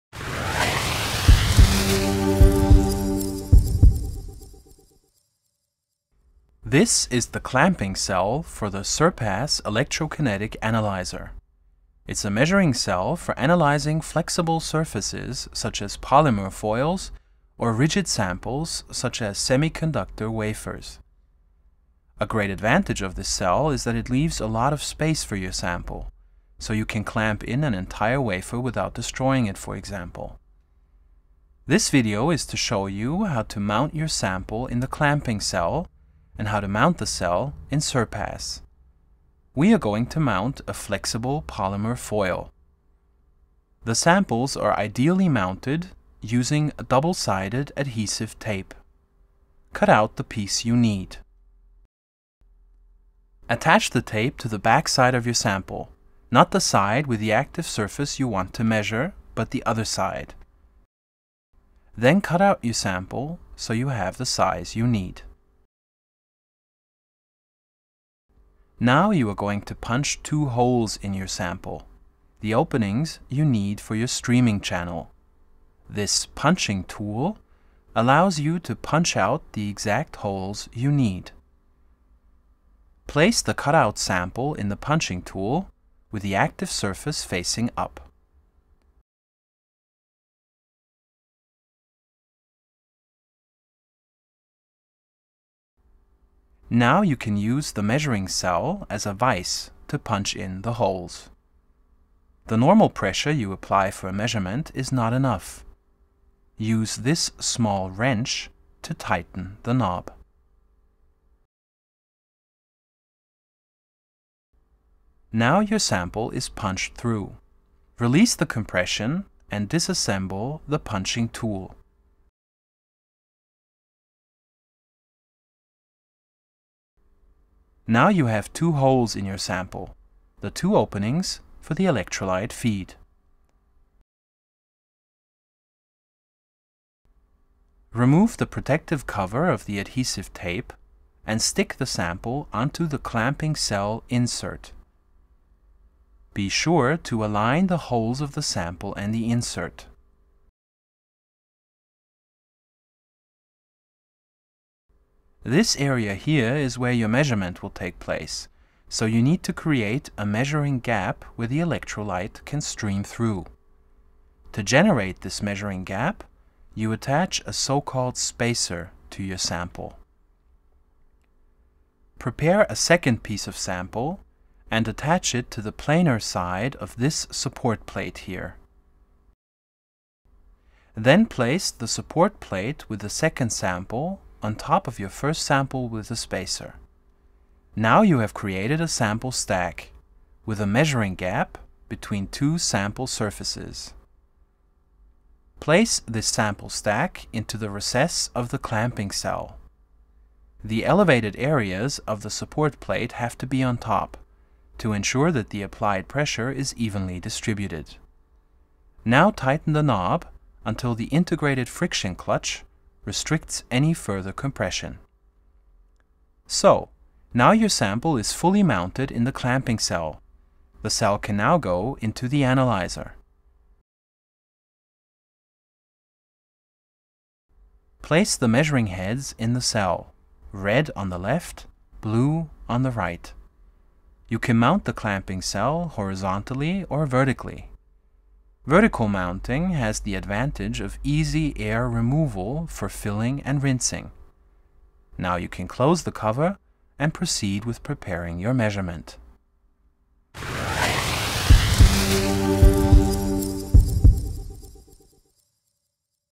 SurPASS: Tutorial on Clamping Cell
This tutorial shows you how to prepare and mount the Clamping Cell in the SurPASS electrokinetic analyzer. This cell is ideally suited for analyzing flexible surface such as polymer foils, or rigid samples such as semiconductor wafers.